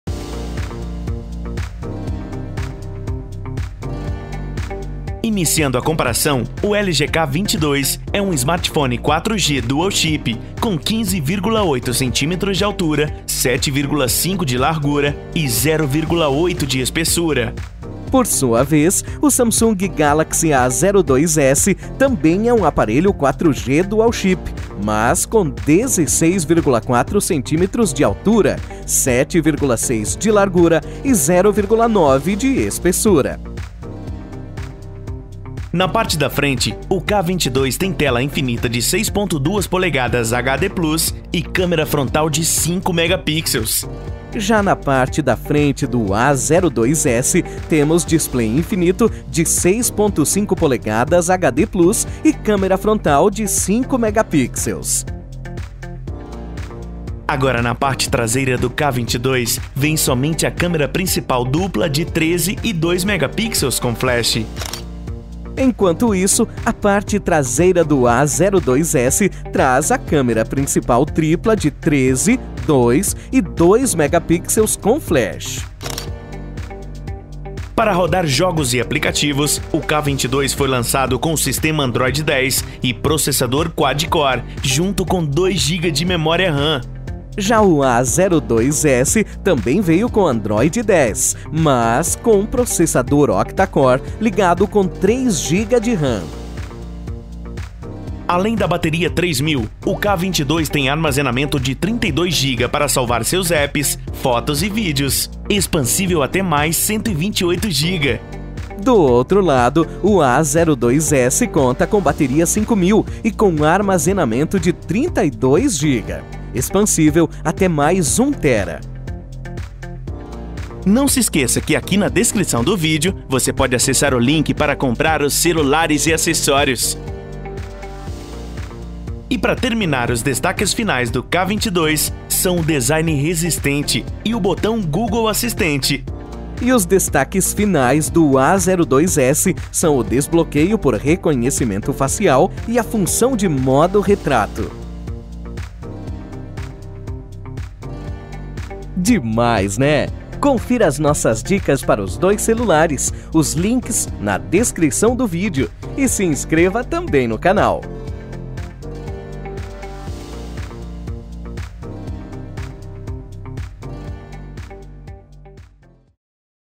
LG K22 vs A02s (Comparativo)
LG Titânio vs Samsung Preto no comparativo entre a ficha técnica do K22 e do Galaxy A02s, + uma comparação de tamanho exclusiva (3D), e muito mais.

Links para comprar:

(LG K22)

Pergunta:
Se o fabricante diz que o smartphone tem design resistente, vocês deixam de usar capinha?